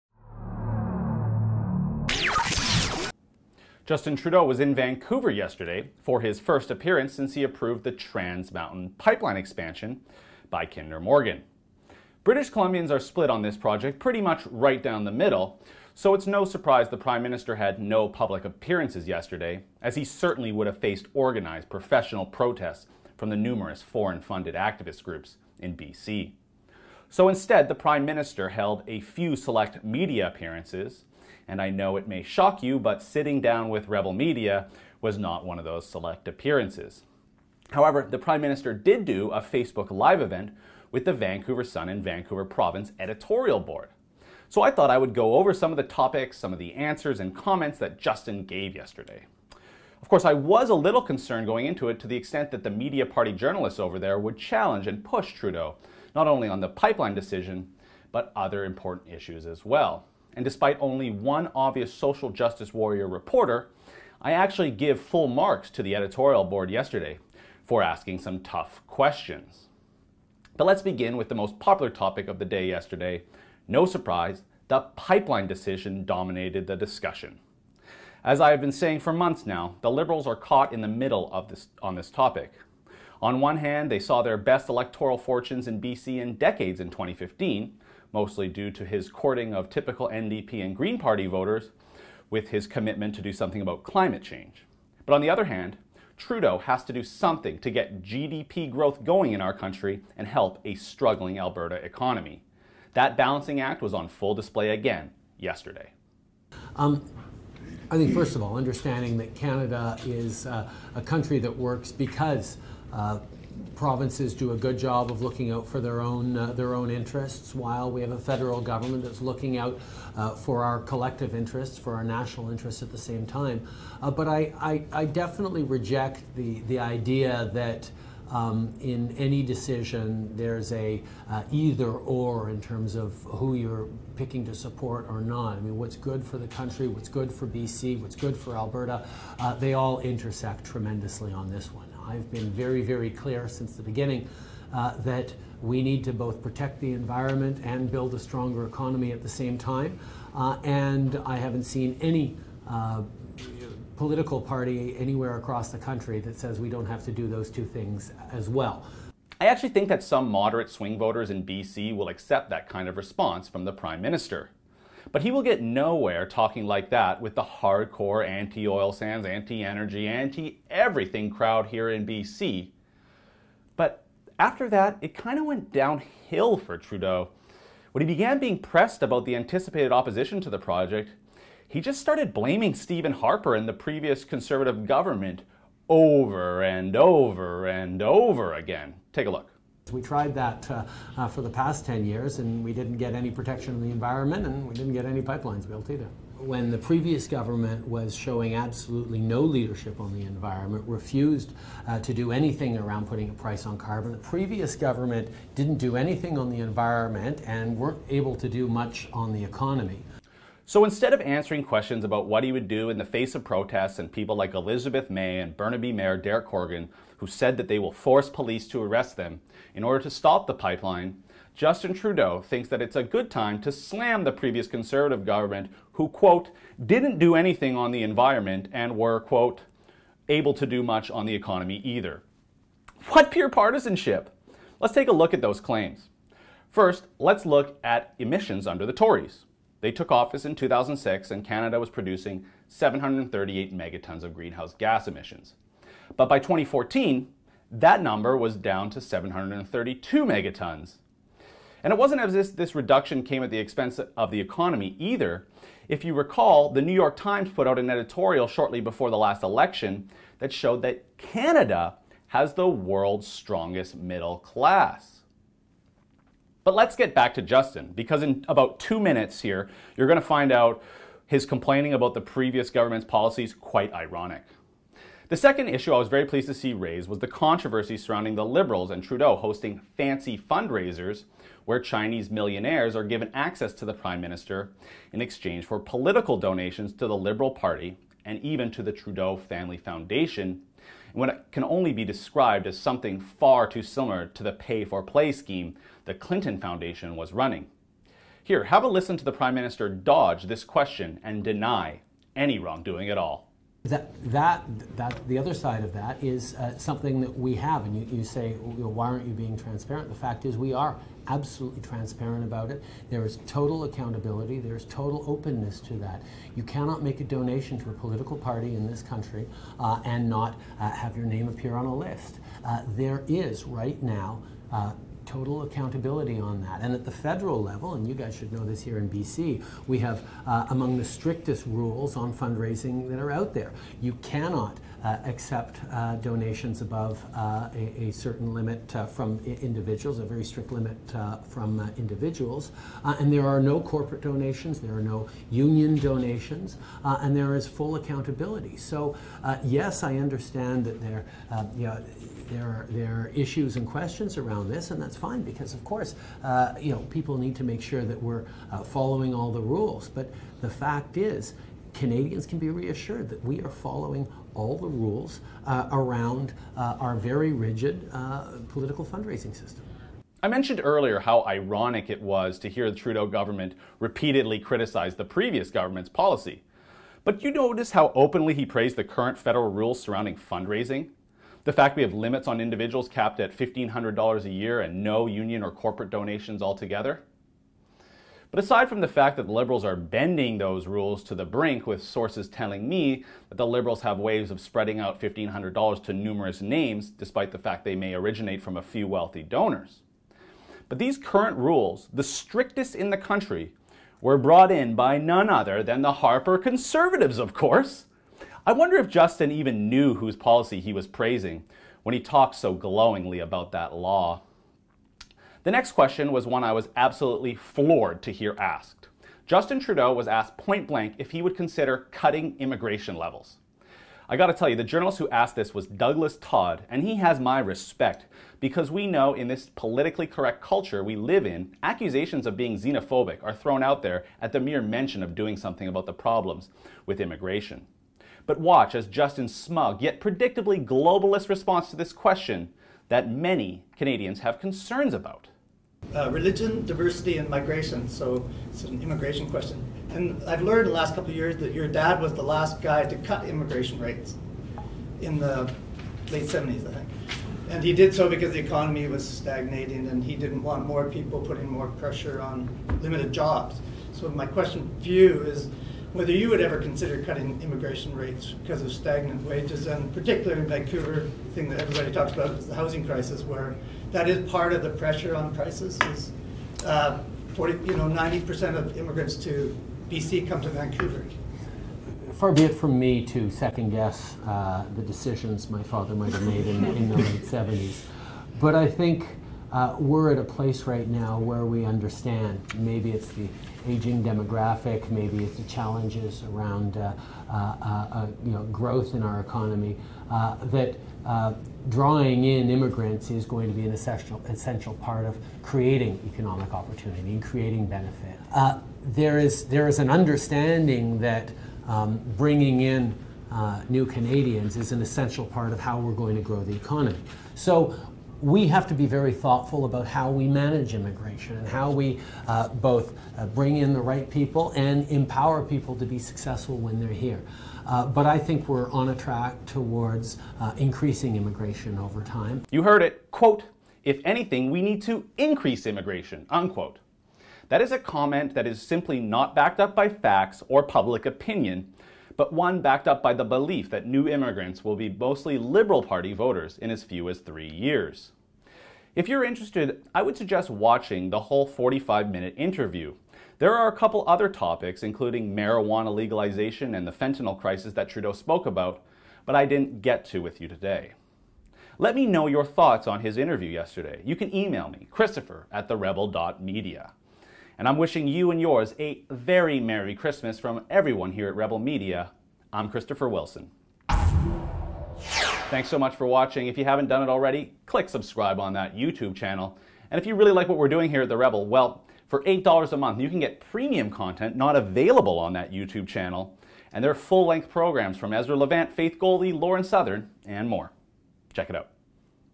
Trudeau in Vancouver: Tough media questions, globalist answers
Christopher Wilson reviews Justin Trudeau's first interview with media in Vancouver since approving the TransMountain pipeline.